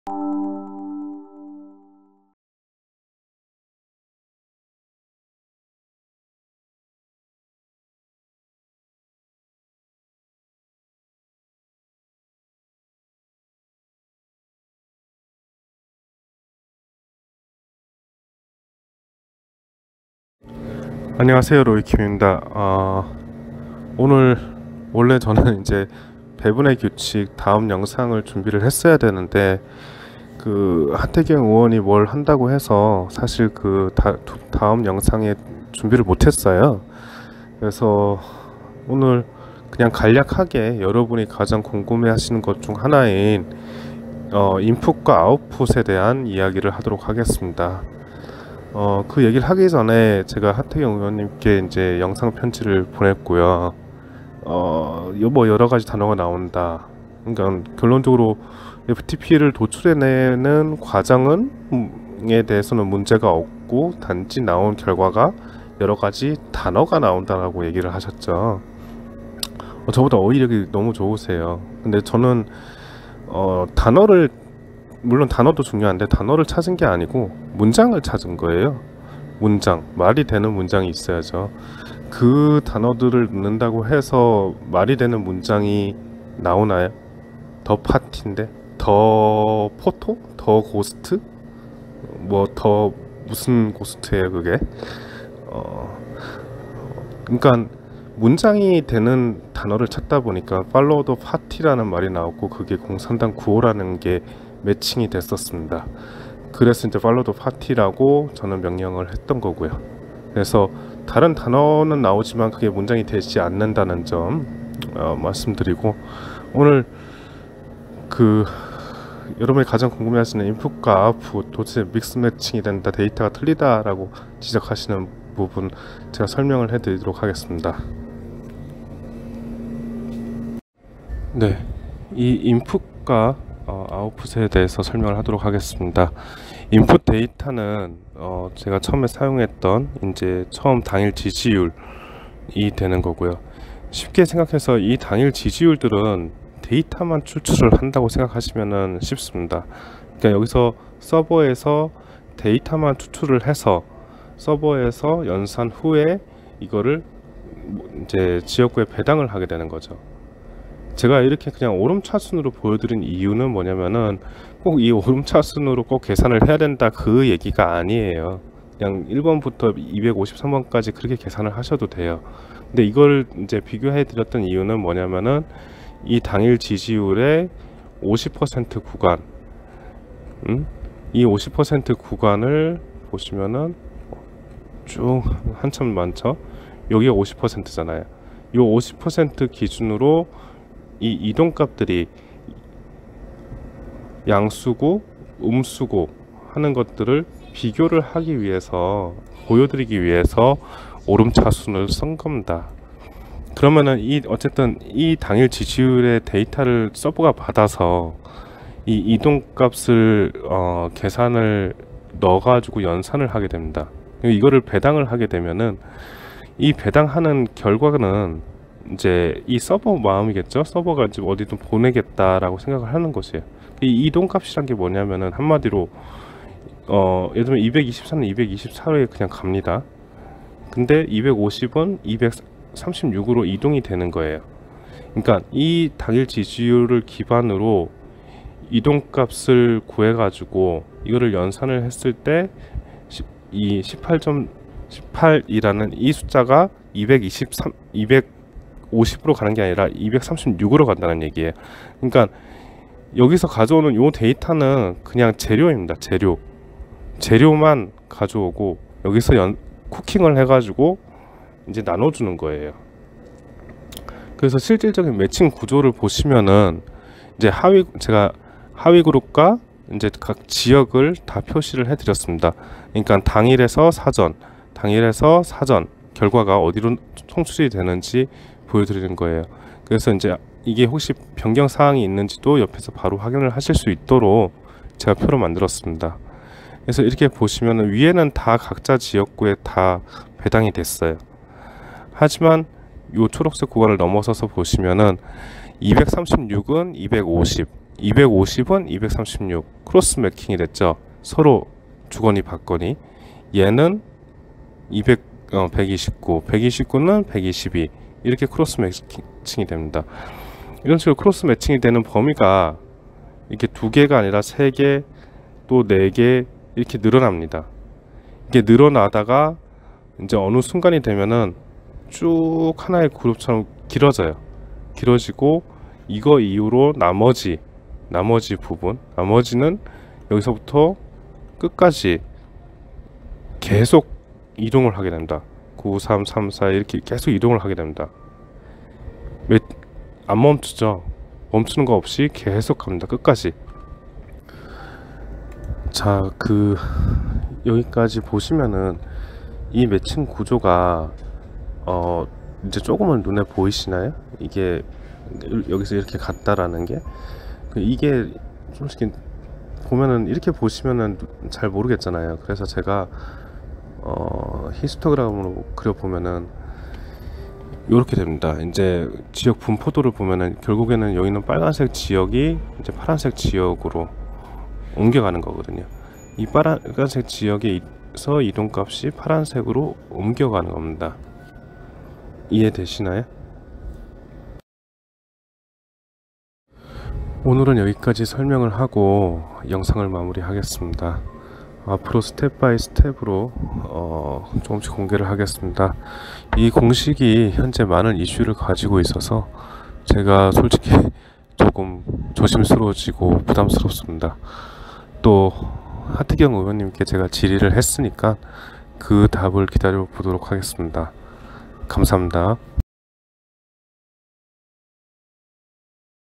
[VON단독] 로이킴의 무편집 직강④ 입력한 선관위 데이터와 산출된 데이터가 다른 이유_매칭 구조 INPUT &OUTPUT 간략 설명
[VON단독] 200601로이킴의 무편집 직강④ 입력한 선관위 데이터와 산출된 데이터가 다른 이유_ 매칭 구조 INPUT &OUTPUT 간략 설명

자율구독 :  하나은행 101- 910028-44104 (주) 브이오엔
국민은행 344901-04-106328 (사)법치와자유민주주의연대